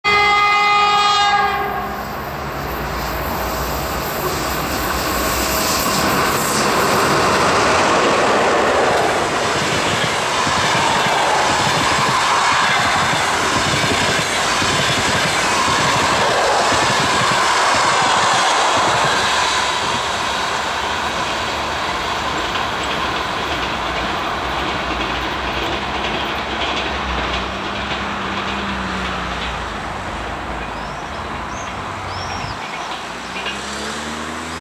ЭП1М-703 с поездом №340 Нижний Новгород — Новороссийск
Электровоз ЭП1М-703 (приписан ТЧЭ-3 Россошь Ю-ВОСТ) с поездом №340 Нижний Новгород — Новороссийск, перегон Развилка - Ростов-Товарный, о.п. Орджоникидзе, Лиховское направление пути, г. Ростов-на-Дону, Северо-Кавказская железная дорога, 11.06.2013 г.